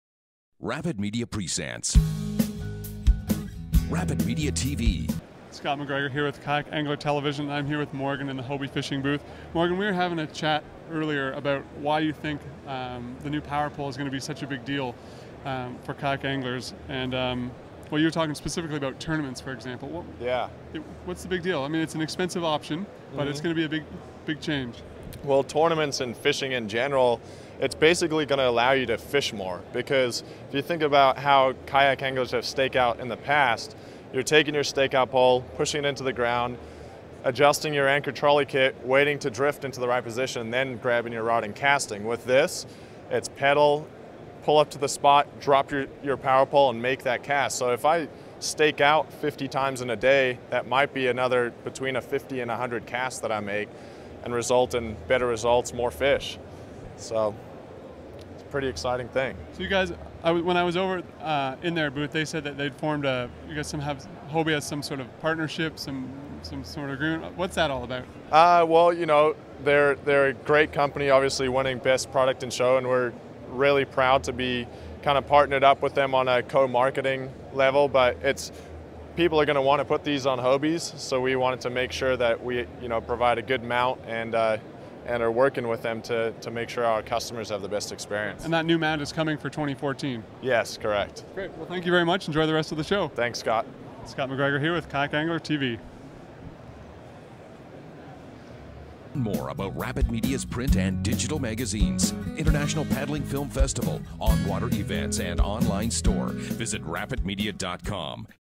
Will Power-Pole Change Kayak Fishing Tournaments?
We talk to pro angler Morgan Promnitz at iCast 2013 on why he thinks the Power-Pole is going to result in better tournament finishes. Will it mean more fish for the everyday kayak angler too?